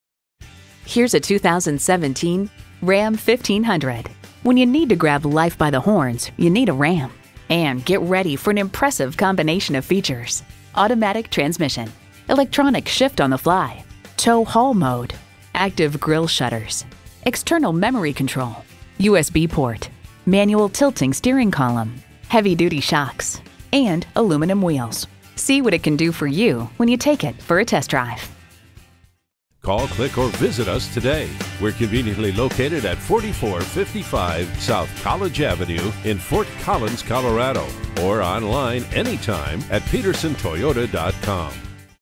Used 2017 Ram 1500 Fort Collins Loveland, CO #1845101 - SOLD
Year: 2017
Make: Ram
Model: 1500
Trim: Express
Engine: 8 Cyl. 5.7 L
Transmission: Automatic
Color: Blue Streak Pearlcoat
Interior: Diesel Gray/Black
Mileage: 6159
Stock #: 1845101
VIN: 3C6JR7AT6HG641096
Recent Arrival!Priced below KBB Fair Purchase Price! CARFAX One-Owner. Odometer is 8424 miles below market average!EXCEPTIONALLY CLEAN AND WELL CARED FOR VEHICLE!!!, ***ONE OWNER***, ***CLEAN CARFAX!!!***, LOCAL TRADE!!!.

Address:
4455 S. College Ave